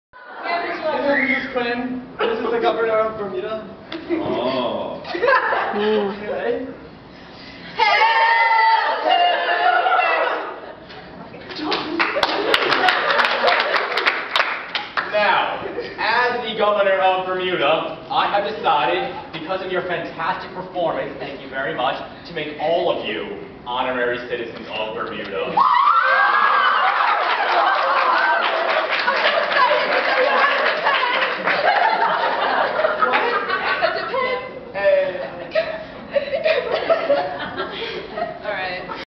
Quinn Razorbacks Best that could happen on Bermuda Tour skit
Poor Cara - she got so excited she wet her plants... tee hee